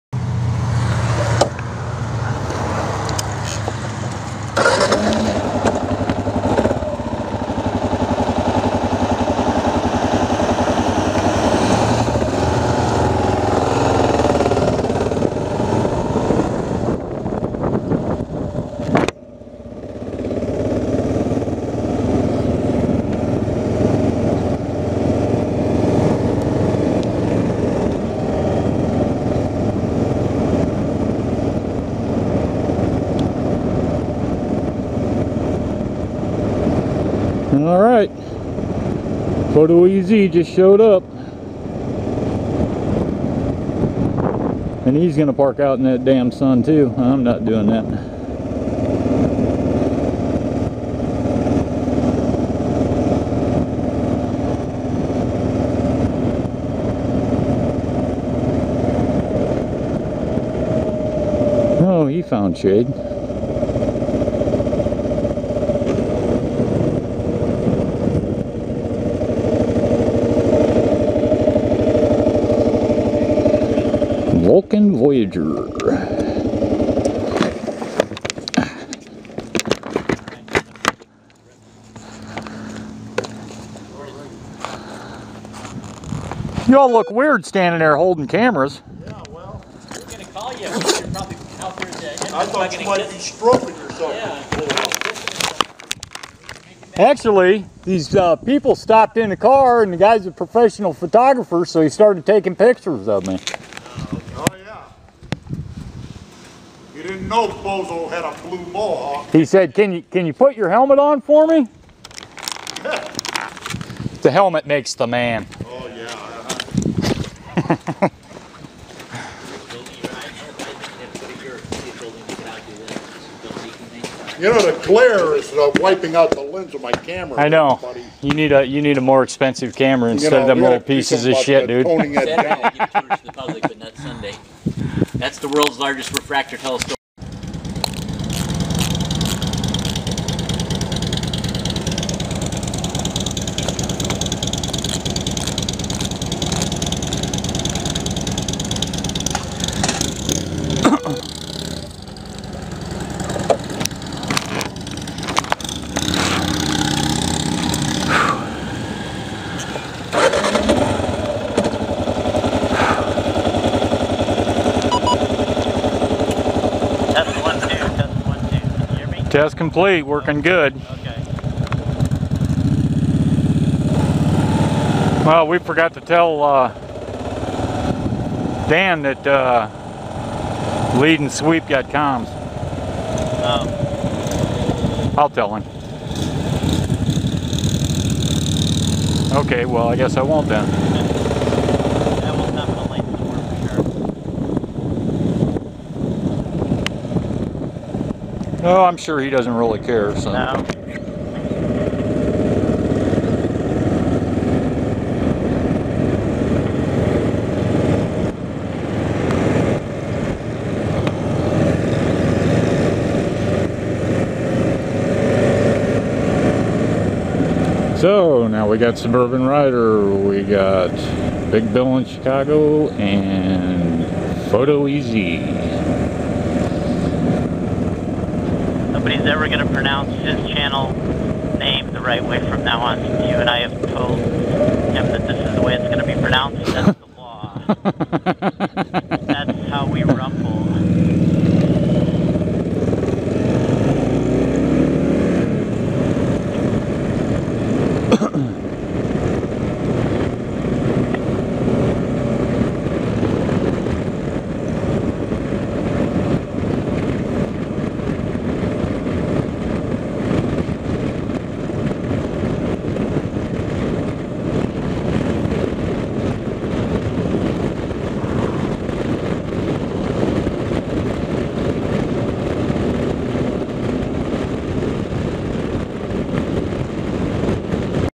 Meet-up with FotoEZ.MP4
had the camera off for the Initial Meetup. so this is a try to catch it vid. have another meetup later this week, with him.